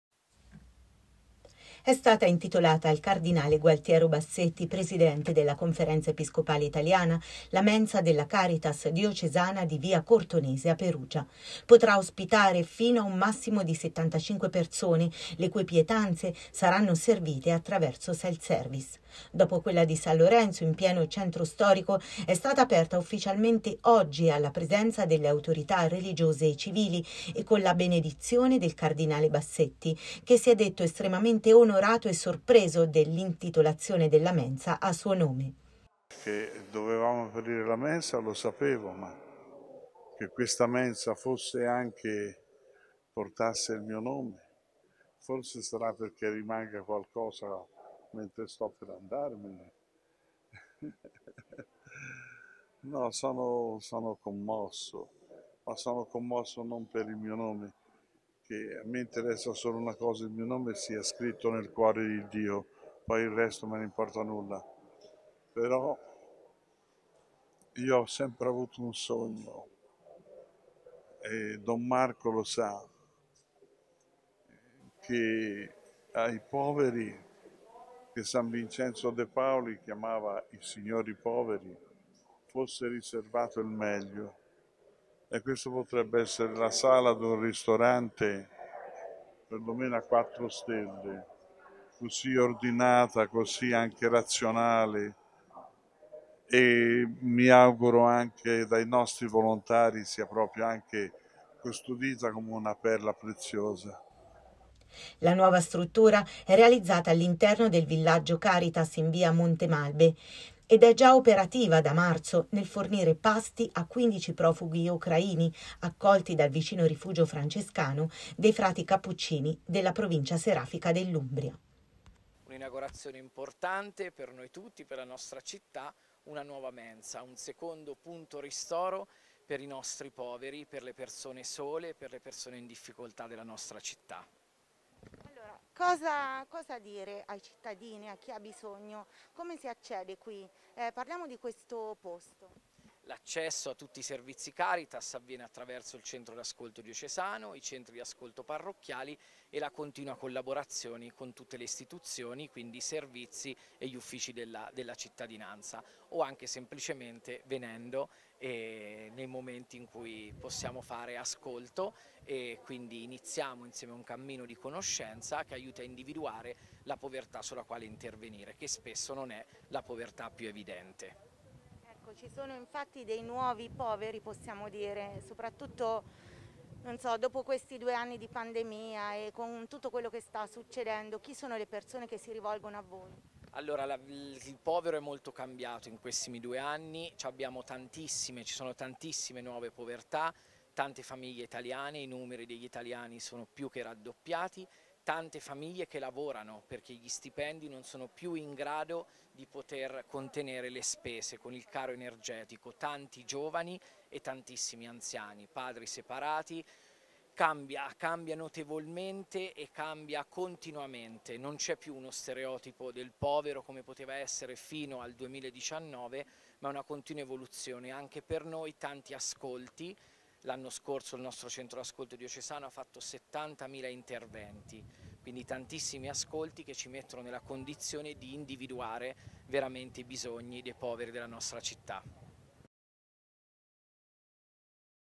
Perugia: inaugurata nuova mensa Caritas intitolata al cardinale Bassetti
Da oggi, venerdì 29 aprile 2022 , sarà operativa la seconda mensa della Caritas diocesana a Perugia. Dopo quella di San Lorenzo in pieno centro storico sarà aperta ufficialmente la nuova struttura realizzata all’interno del Villaggio Caritas in via Montemalbe (zona Cortonese) ed è stata inaugurata mezzogiorno dal cardinale Gualtiero Bassetti.